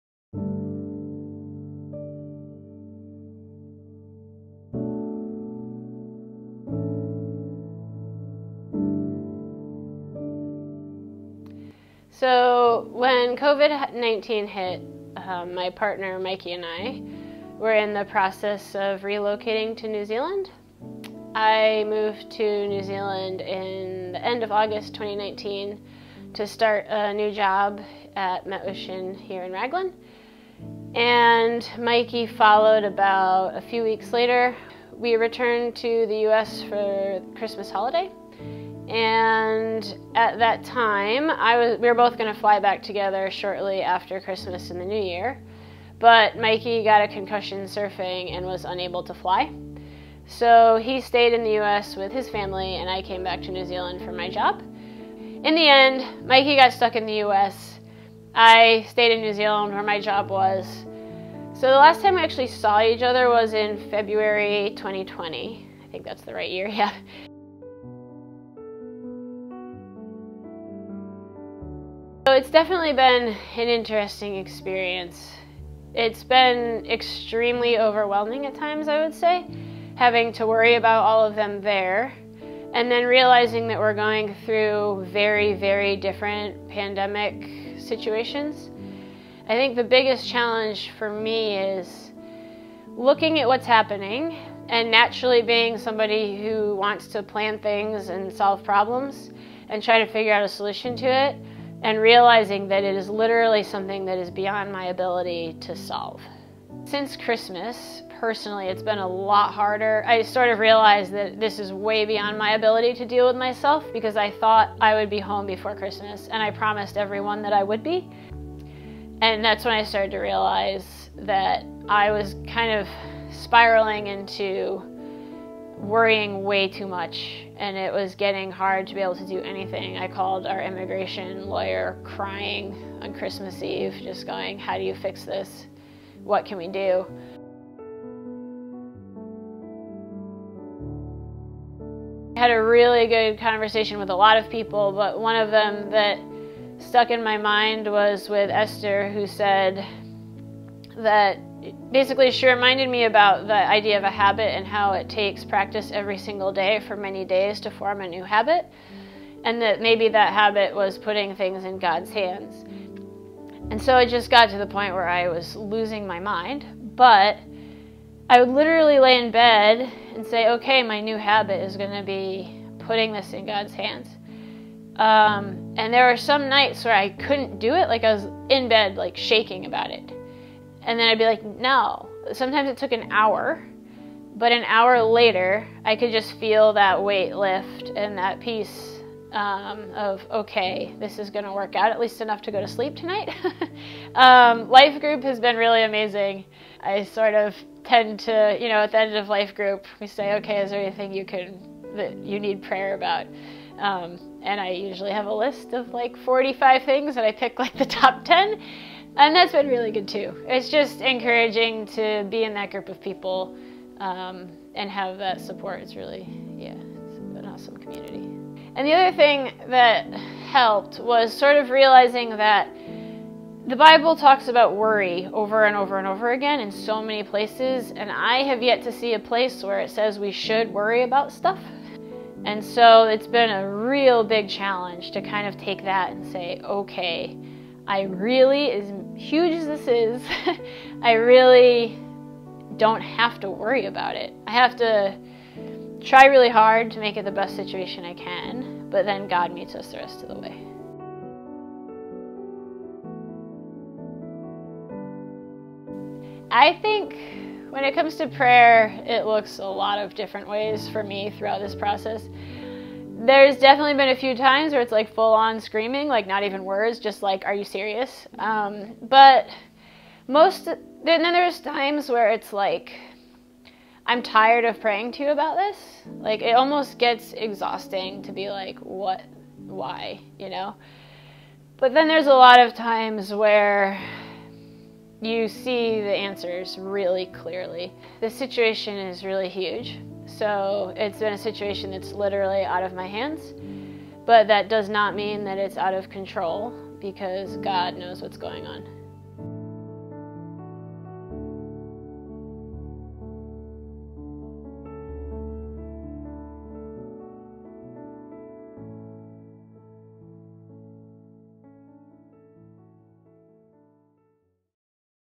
Julie's story
Julie Jakoboski sits down to talk to us about the challenges she's been through during Covid-19 as partner, Mikey, and her have endured separation brought about by lockdowns and border shutdowns for a year and a half.